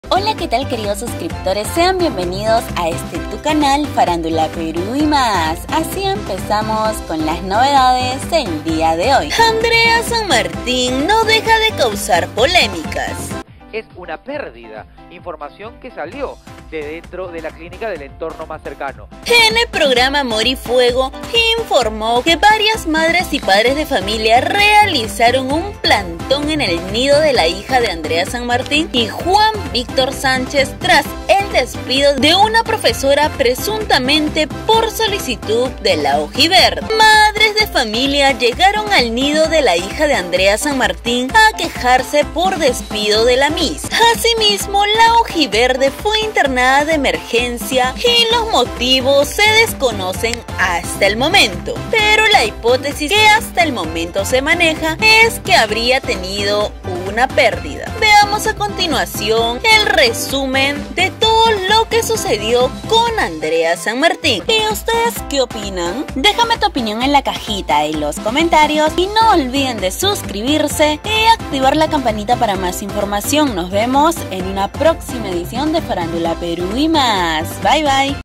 TERRIBLE ANDRE SANMARTIN PROTAGONIZA DISCUCION CONPADRES DEL NIDO DESU PEQUEÑA YLLEGAHASTAELHOSPITAL
DE NO CREER ANDREA SAN MARTIN PROTAGONIZA UNA DISCUCION CON LOS PADRES DEL NIDO DONDE ESTUDIA SU PEQUEÑA HIJA SOLO PORQUE EL PADRE JUAN VICTOR SANCHES SUBIO UNA FOTO DONDE SALE LA PEQUEÑA Y ADEMAS ANDRE SERIA LA CULPABLE DE QUE MAESTRA HAYA SIDO DESPEDIDA FUERTE !